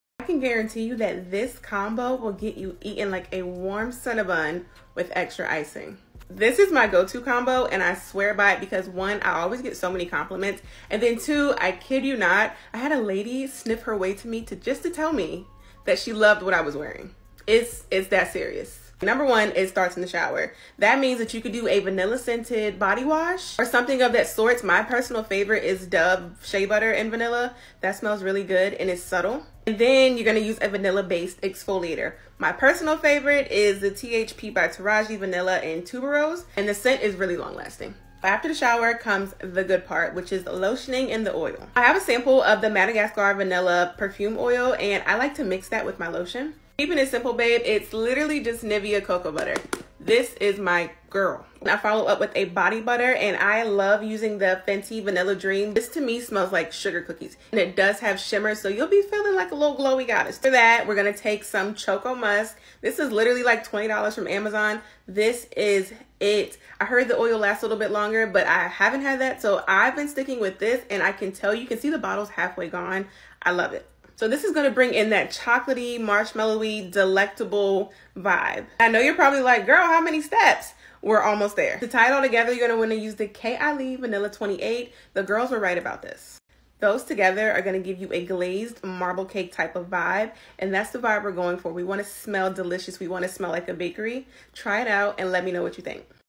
Spring/Summer Must Try Vanilla Fragrance Combo, You'll Smell Irresistible😍🧁
Hey ◡̈ This combo is perfect for the gourmand fragrance lovers ☺️